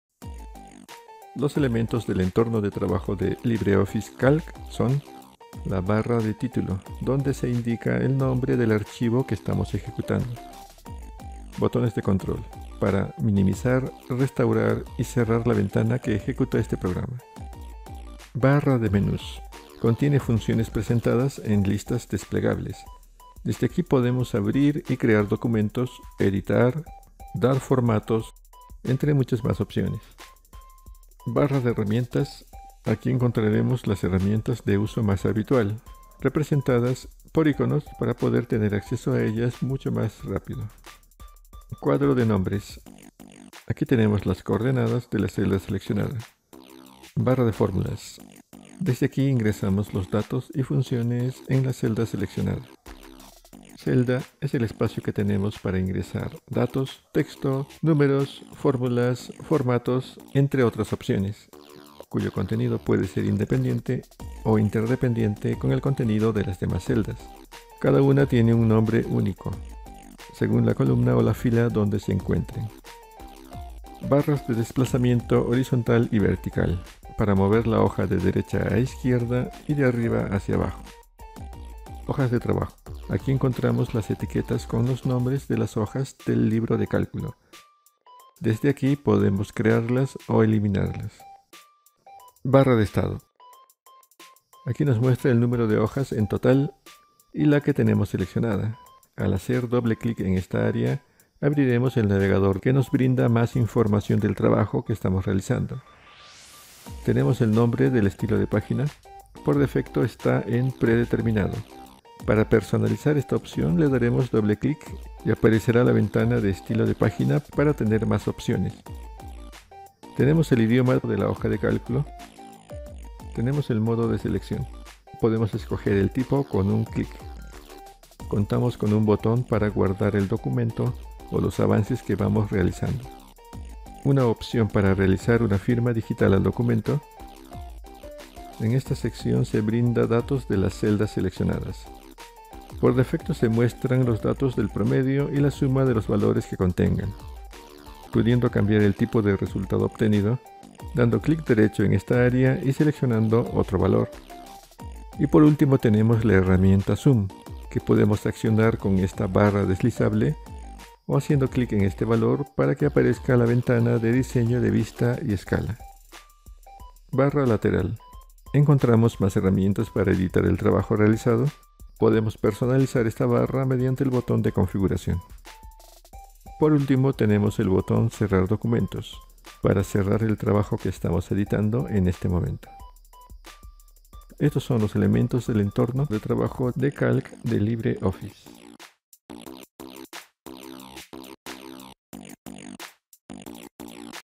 Entorno de trabajo – LibreOffice Calc (curso en línea)
El entorno de trabajo de LibreOffice Calc se divide en diferentes partes, como la barra de menús o la barra de herramientas. En este tutorial, veremos las definiciones de cada uno de los elementos presentes, en el entorno de trabajo de una hoja de Calc.